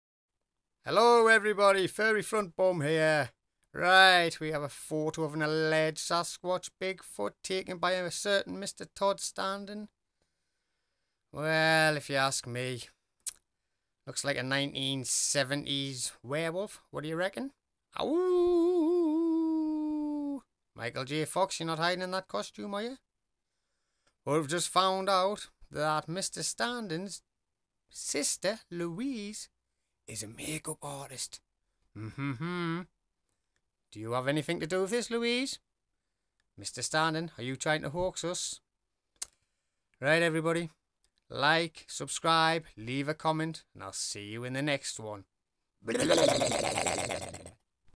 BIGFOOT FACT/FAKE ? You decide (2)
photo of a bigfoot by todd standing. fact/fake ? you decide.